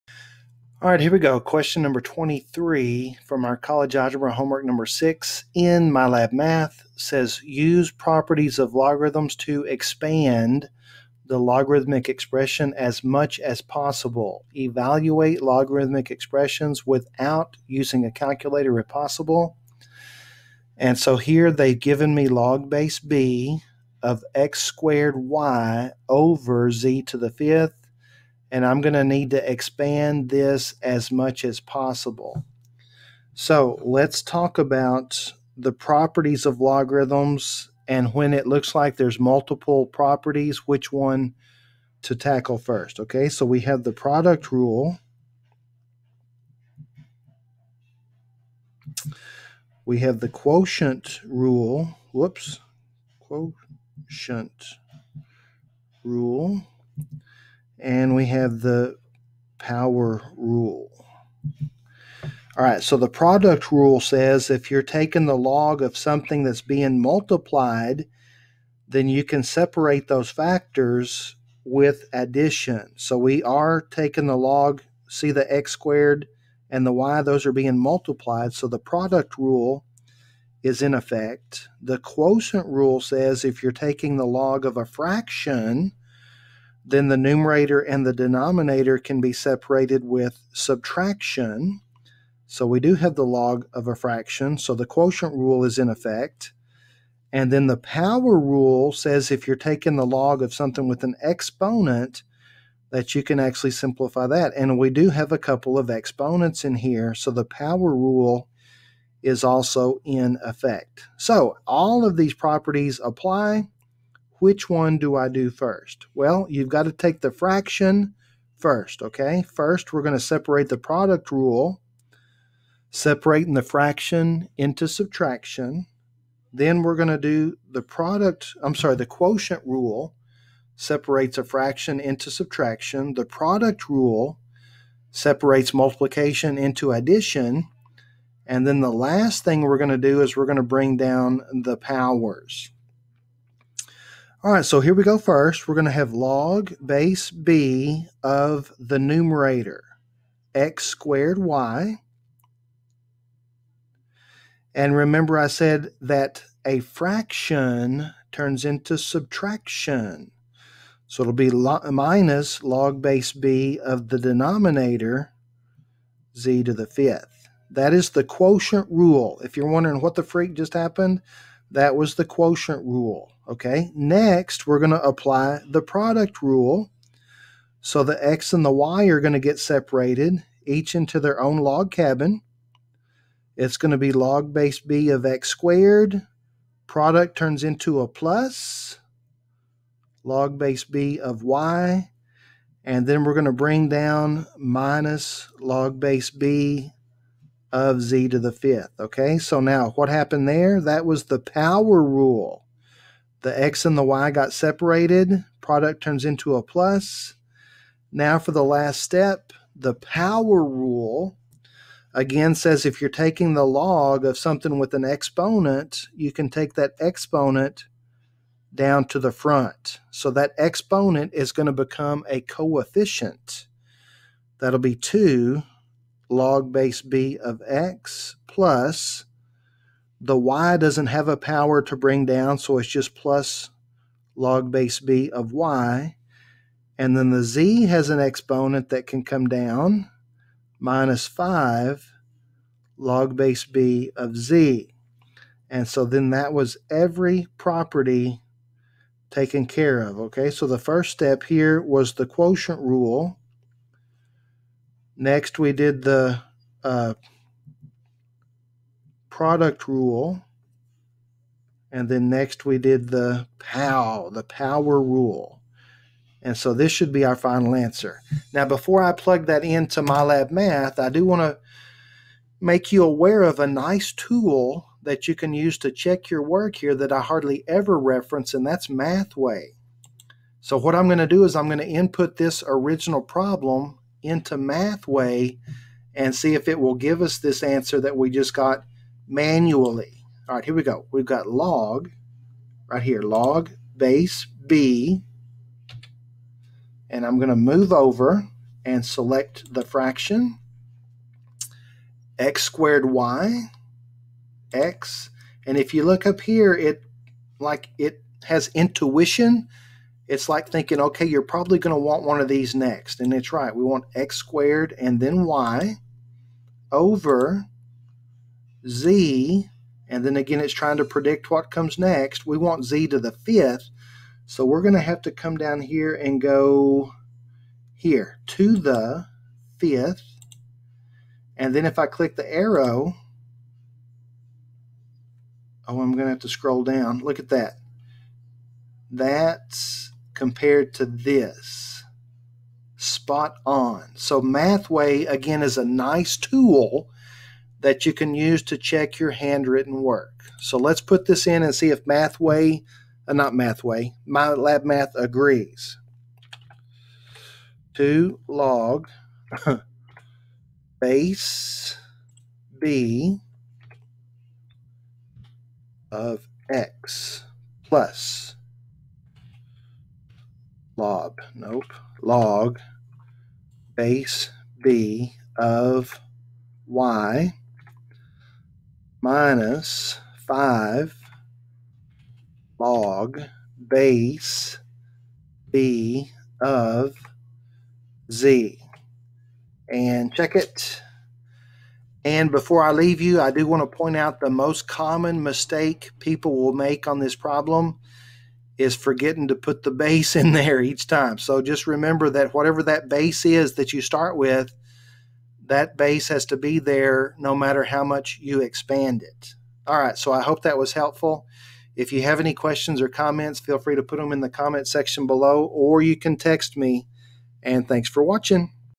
College Algebra - MyLab Math - HW#6: Question #23
Expand a logarithmic expression as much as possible using the properties of logarithms.